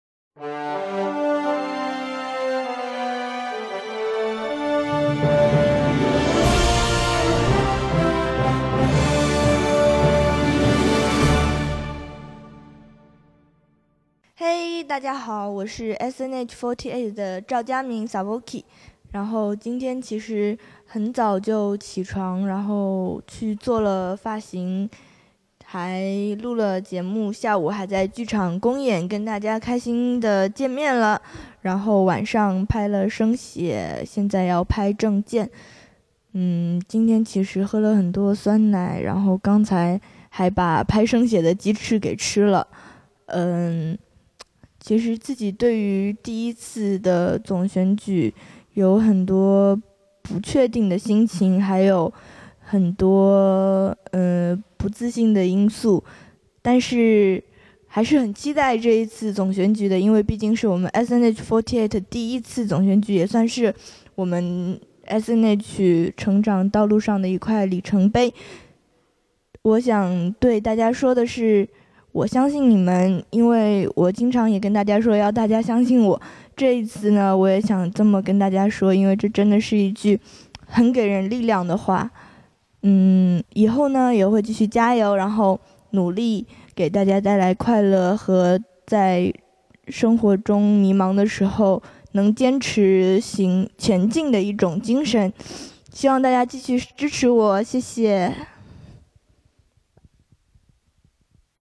SNH48 SII 赵嘉敏 Zhao Jiamin - Appeal Video
SNH48 Team SII member Zhao Jiamin's appeal video for SNH48's 1st General Election!

SNH48第一届总选举"一心向前"宣言 赵嘉敏